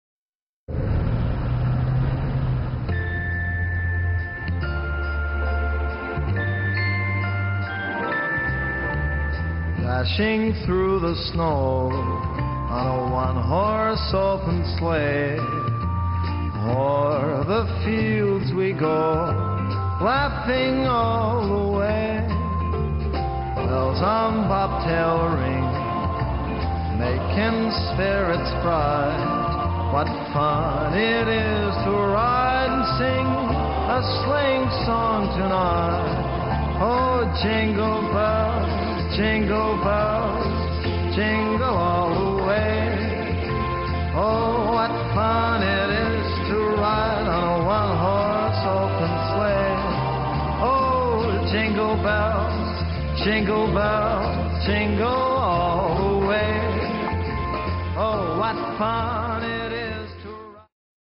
Taxi
que se passe t'il la nuit du reveillon de noel dans les taxis Londoniens